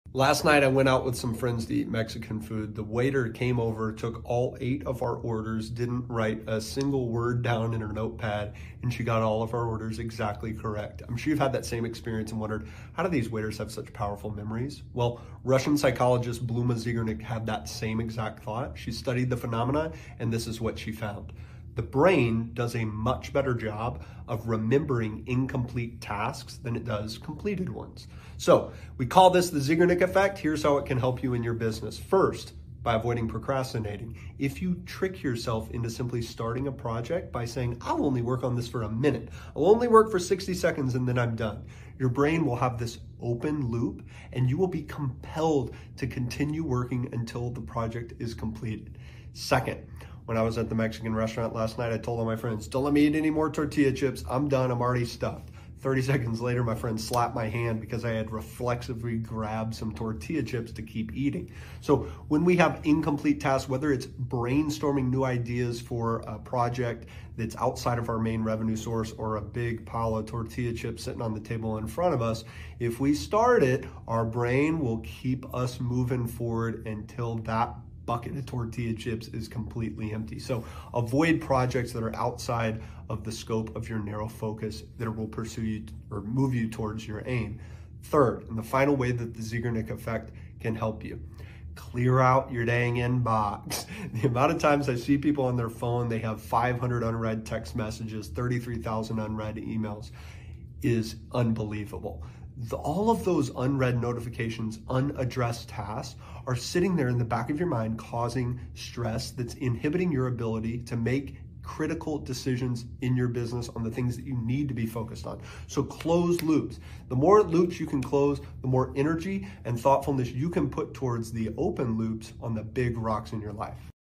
Mental clutter clean-out | DECIDEDLY PODCAST | Sanger D. Smith
The brain does a much better job of remembering incomplete tasks than it does completed ones…we call this the Zeigarnik Effect. 🧠

💭 Here’s how it can help you in your business:

1 – Avoid procrastinating! If you can trick yourself into starting a task, your brain will have an open loop, which compels it to continue working until the task is completed. 🔁

2 – Keep projects that align with your primary goals top of mind, and your brain will narrowly focus to align your energy with accomplishing them. 📎

3 – Clear out your inbox! 📩 Your brain wastes energy on unaddressed tasks. Unopened emails inhibit your ability to make critical decisions because you’re subconsciously focused on open loops like a full inbox.

The more loops you can close, the more thoughtfulness you can give to the important decisions in your life.